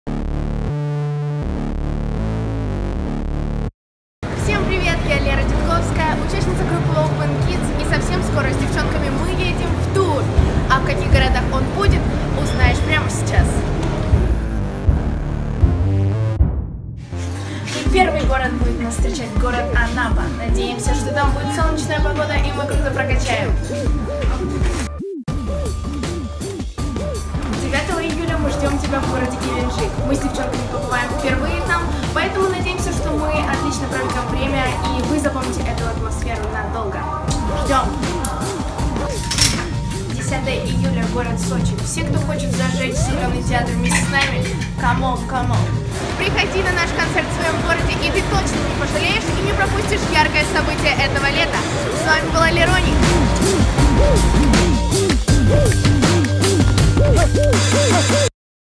Open Kids - Валерия Дидковская - Приглашение на сольный тур - Анапа - Геленджик - Сочи
Кидсеры, уже совсем скоро Open Kids едут в тур! И участница группы Лера Дидковская приглашает всех не пропустить это яркое событие! Давайте прокачаем лето 2016 вместе!

08/07 Анапа - Летняя Эстрада
09/07 Геленджик - ГДК
10/07 Сочи - Зеленый театр

Уже открыты продажи билетов на большой сольный концерт Open Kids! Только живой звук и живые эмоции, не пропустите!

Организация концертов на территории России: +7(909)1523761  Виктория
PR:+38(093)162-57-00